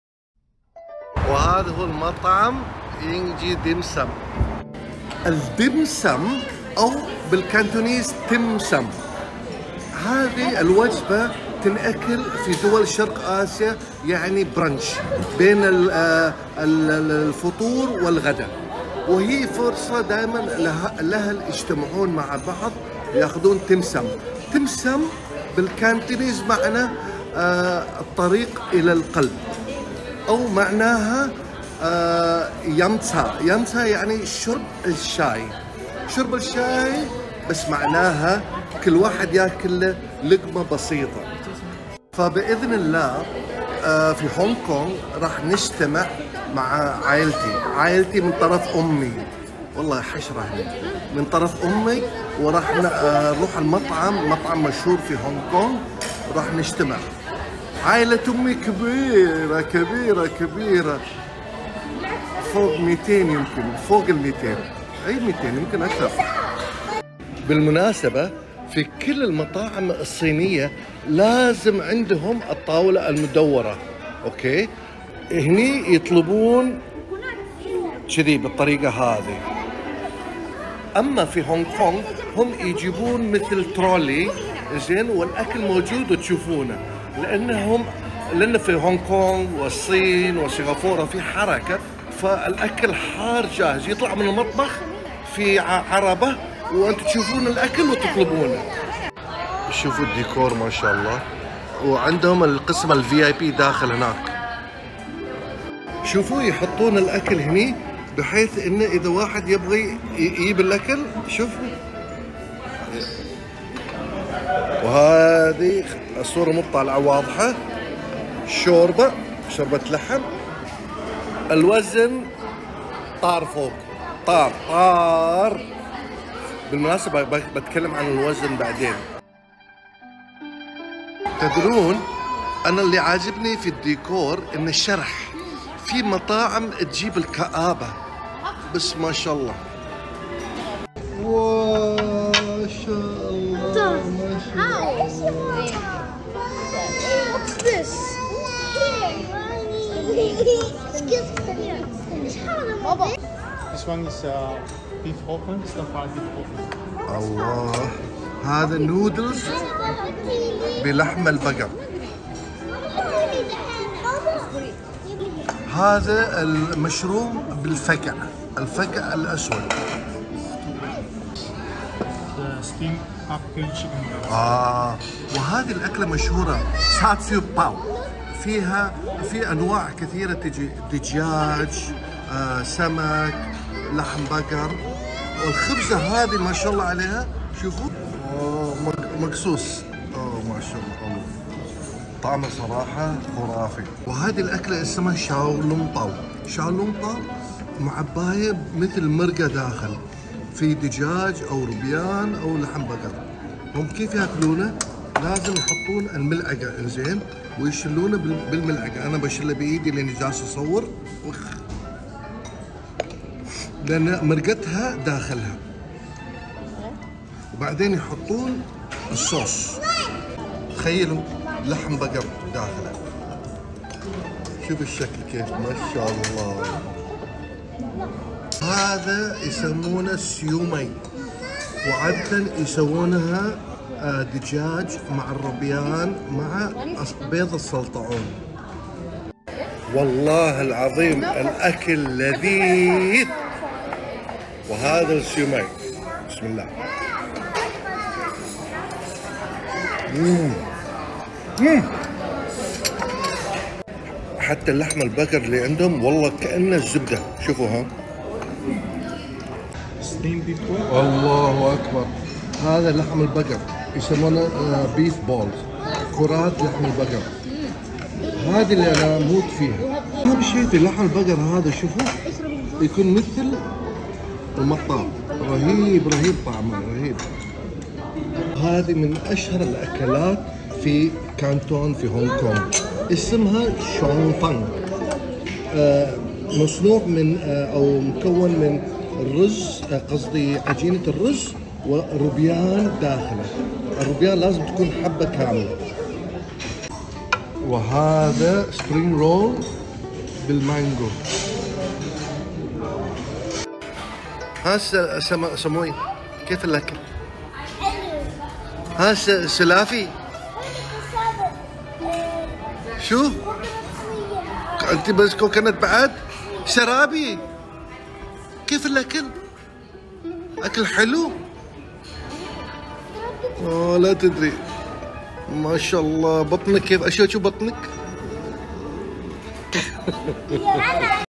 مطعم YING JI في مول دبي و نبدة عن وجبات الديم سم dim sum و شو معناها
فولو ولايك و تفعيل التنبيهات ( 🔔 ) عشان توصلك الفيديوهات الجديده